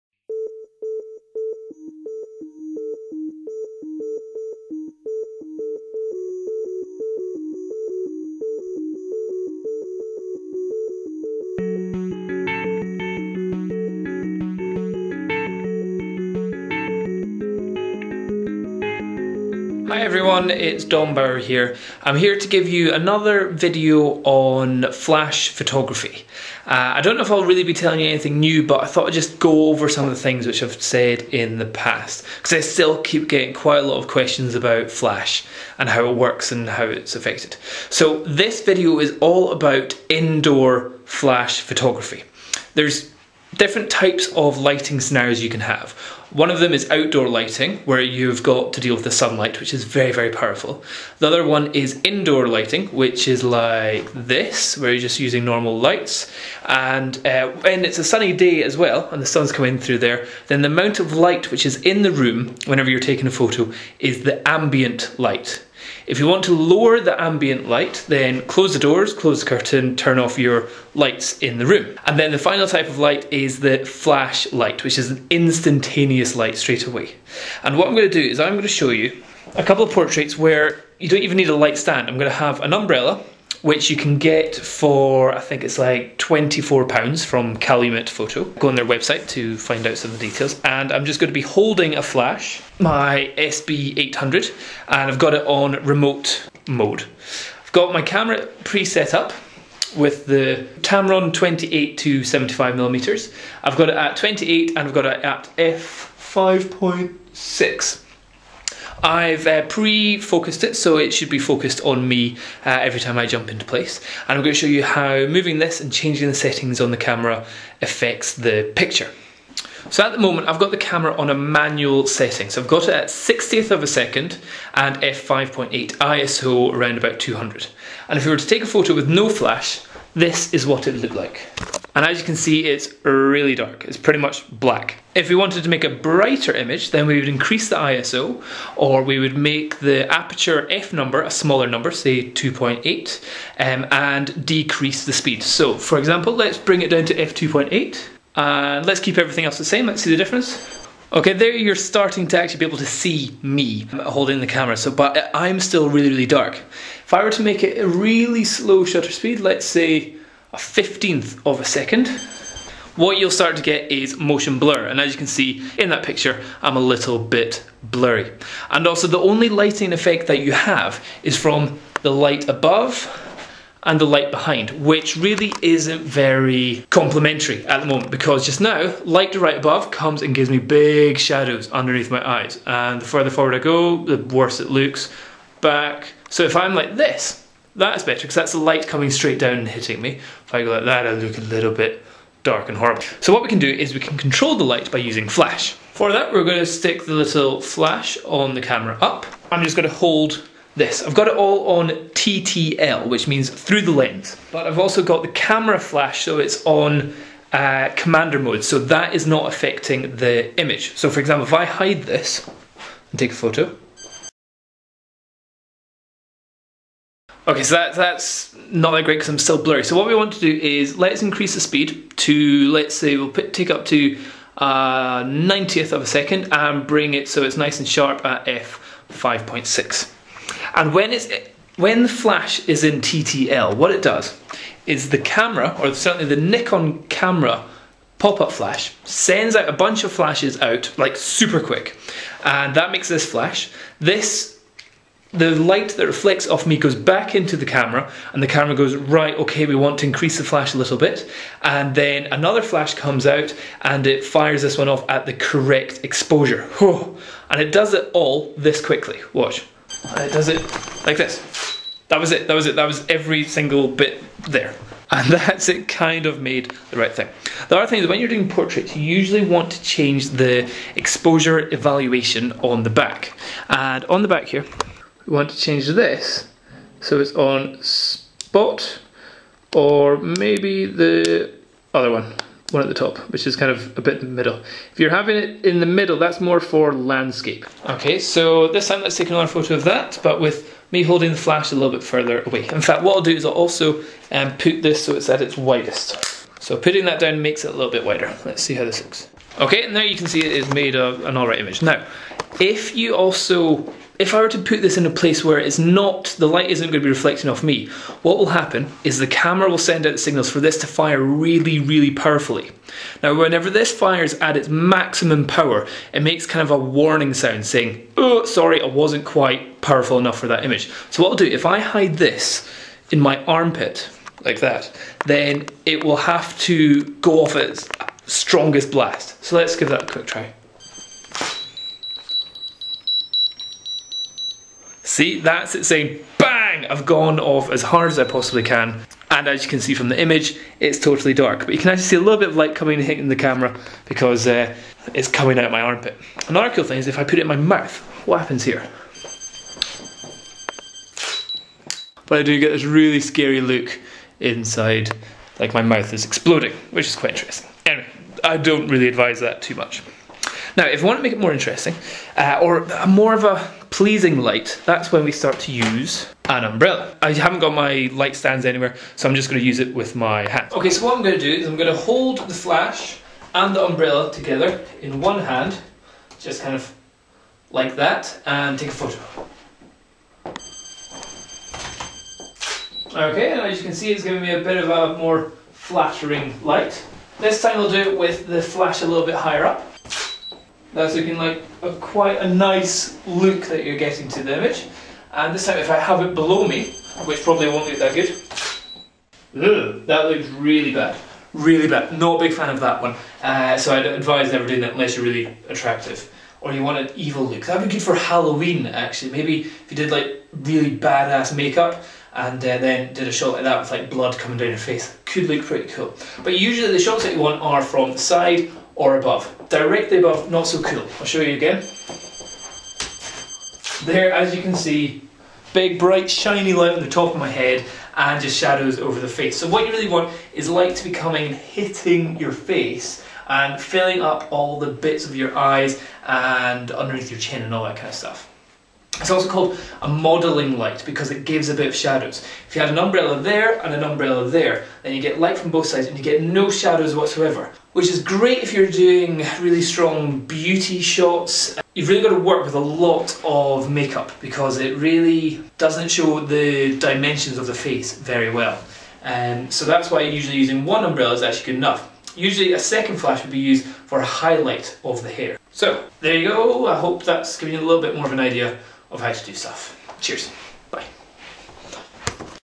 More Flash Photography Tips (Dom Bower) (October 2009)
some more tips about flash photography by Dom Bower